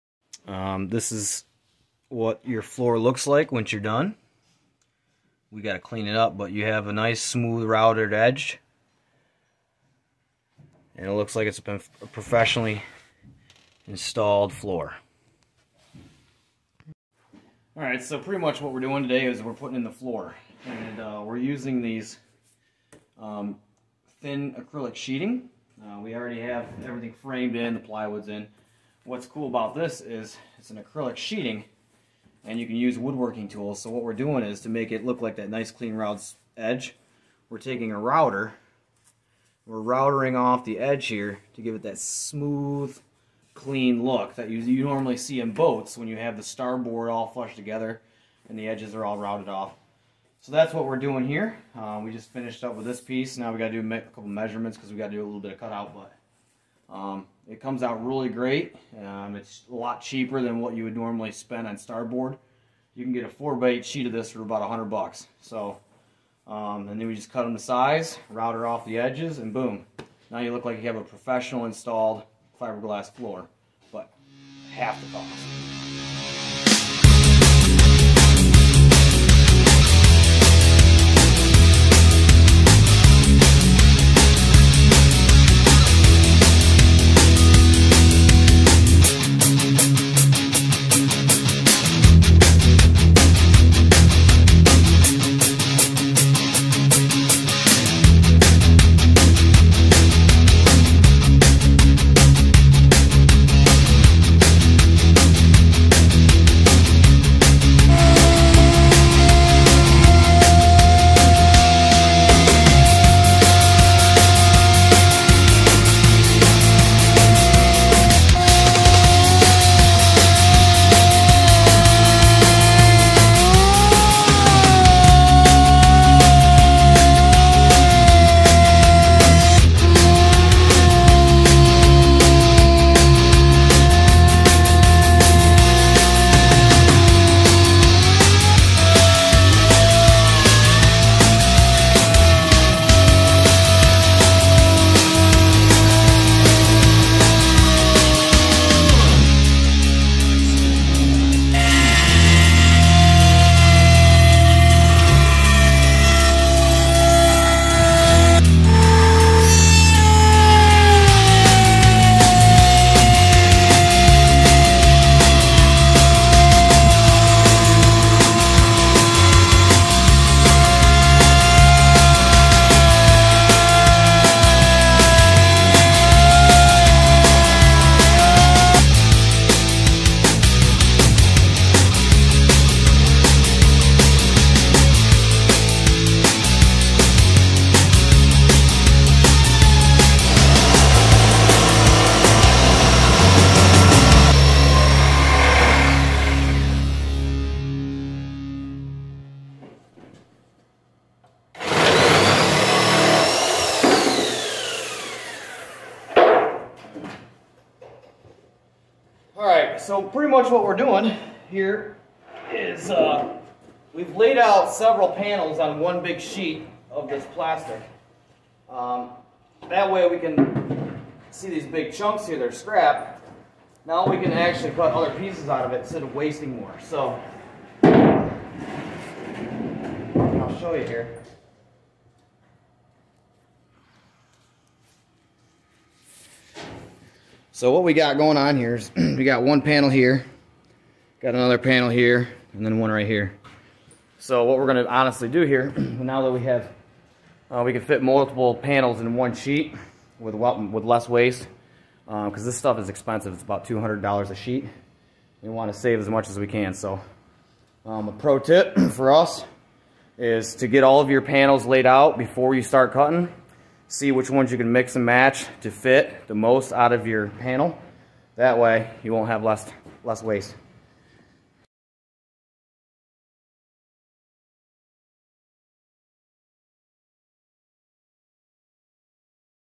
1989 Crestliner Sabre overhaul (plastic wood installation, helm, floor, and boat progress)
Giving you all an update on the boat progress. It's been a long winter but the boat is starting to take shape. We have complete rebuilt the interior. We added all new electronics from RayMarine, added speakers, LED lighting, and have replaced the carpet with all new plastic wood recycled from plastic gathered in our waterways.